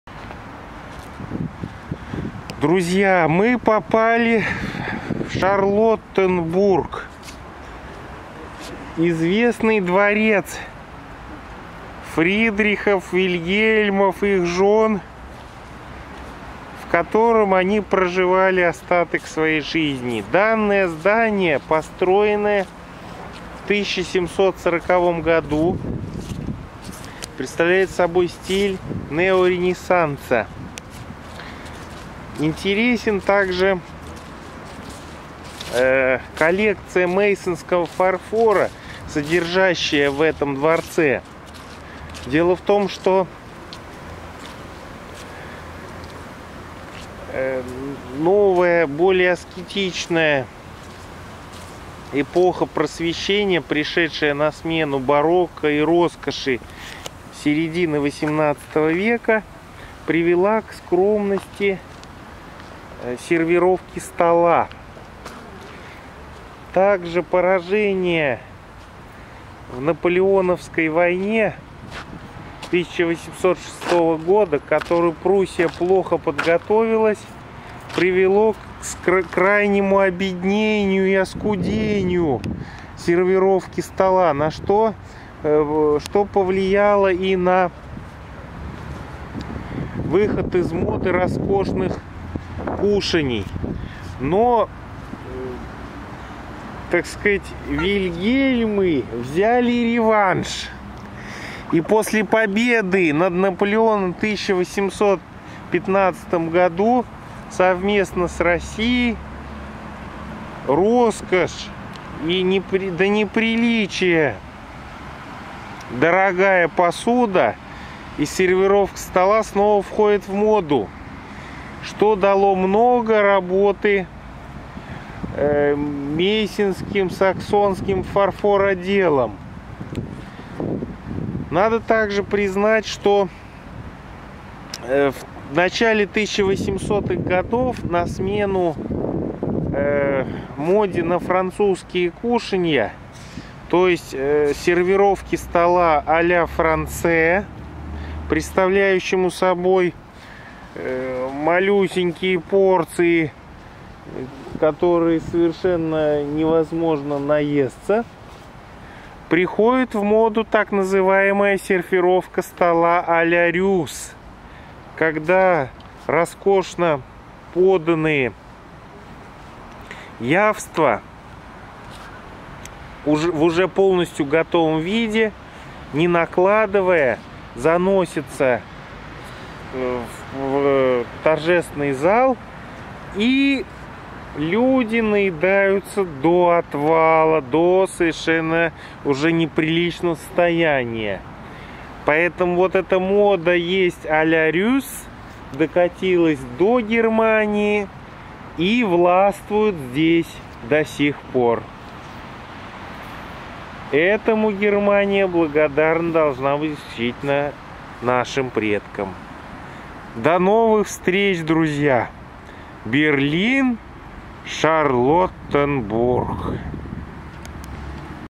Антон Смирнов. Шарлоттенбург.
О еде а"ля Рюс, сервировке стола и Мейссенском фарфоре. Cнято на вдохновении после посещения дворца Шарлоттенбург в г. Берлин, ФРГ, 28 октября 2016 года.